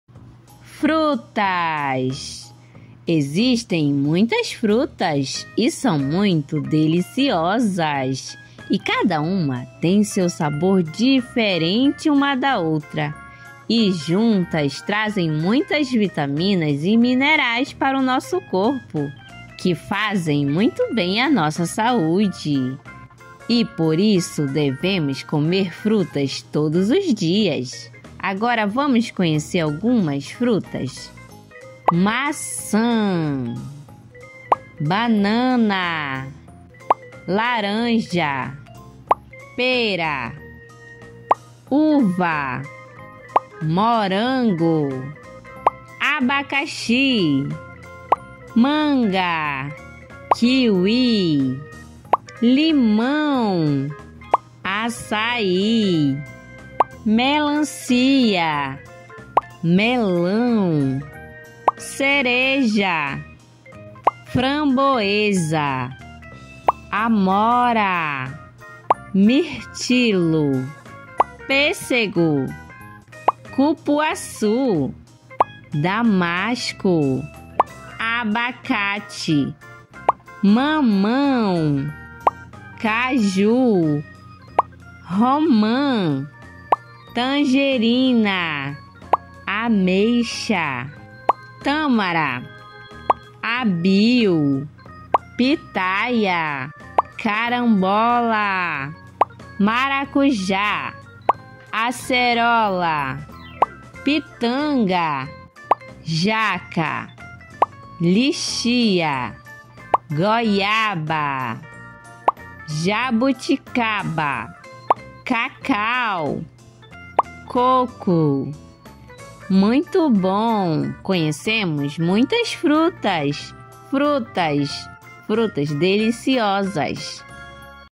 Nomes das frutas para crianças| Vídeo educativo infantil| Vocabulário
Nomes das frutas para crianças. Vocabulário. Aprendendo os nomes das frutas: banana, abacaxi,abacate, lichia,cocô,uva,amora,melancia, melão, açaí,abiu,e muitas outras frutas.
As frutas são saudáveis e fazem muito bem a nossa saúde.